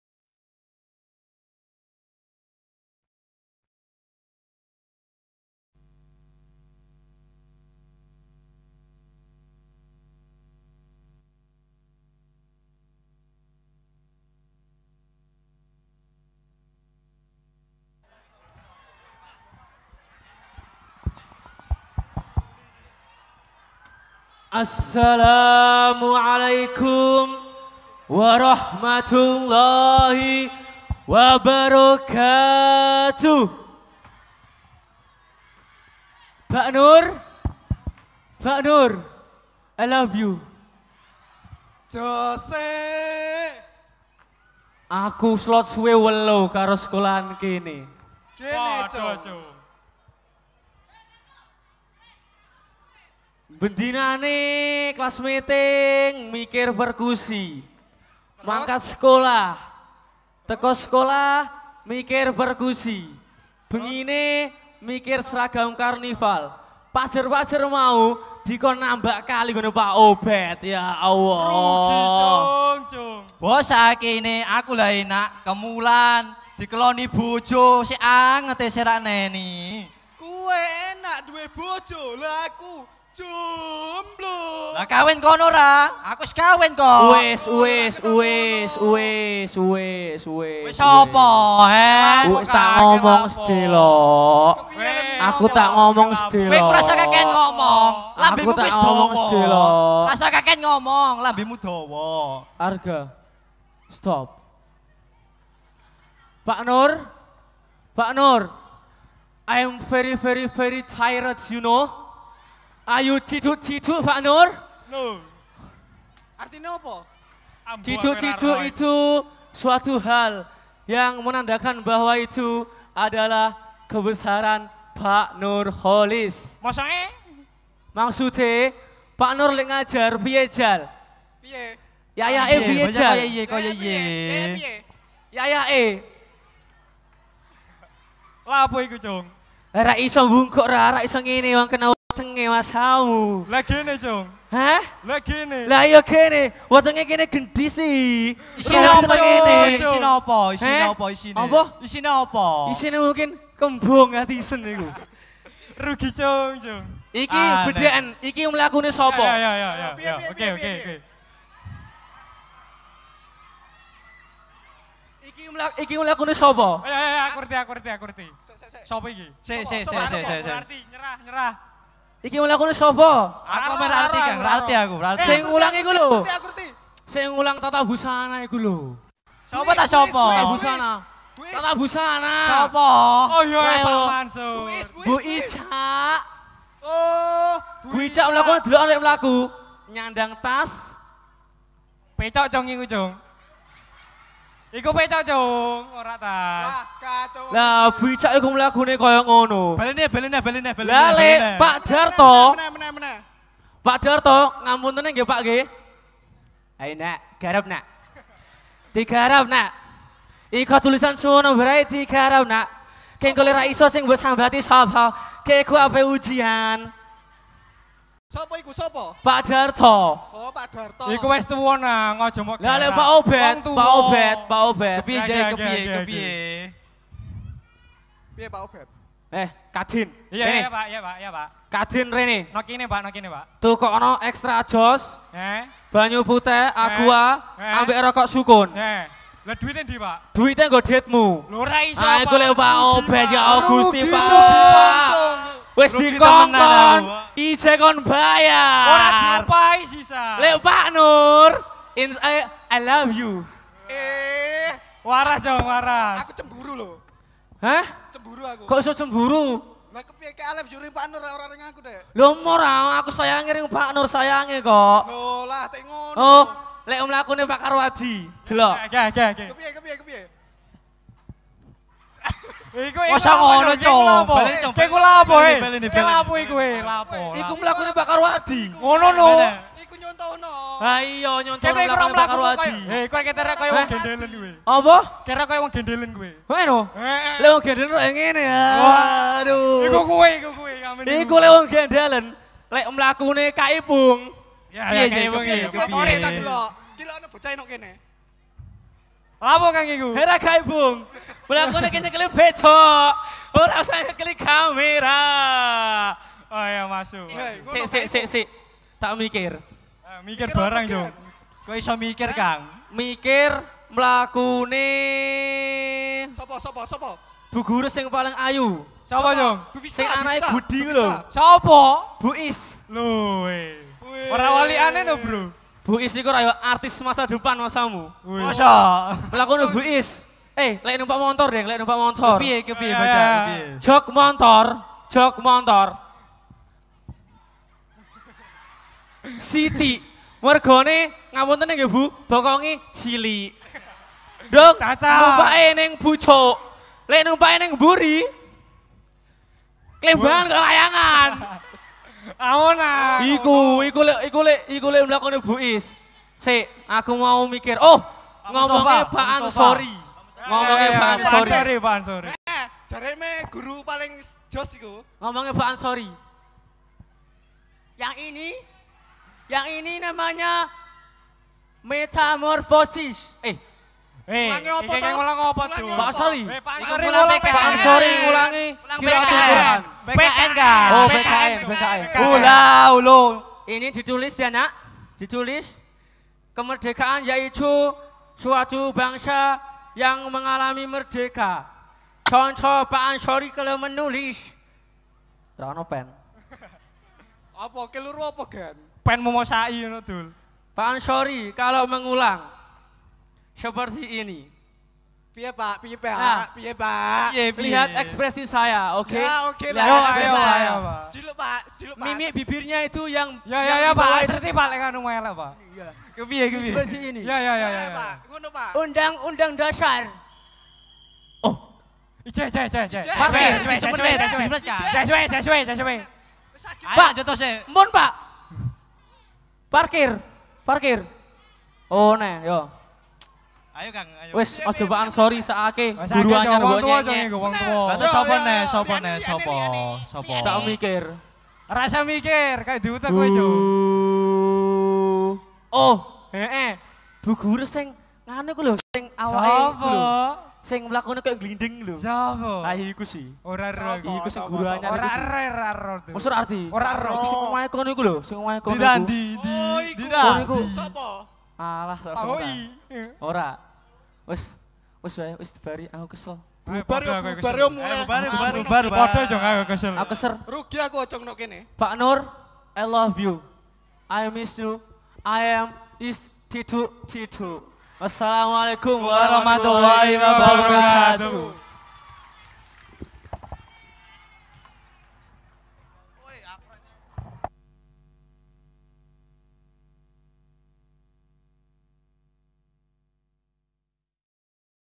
LAWAK XII IPA 1 - MA MATHOLI'UL HUDA TROSO
Lomba Lawak Class Meeting Semester Gasal Tahun 2015 MA Matholi'ul Huda Troso Pecangaan Jepara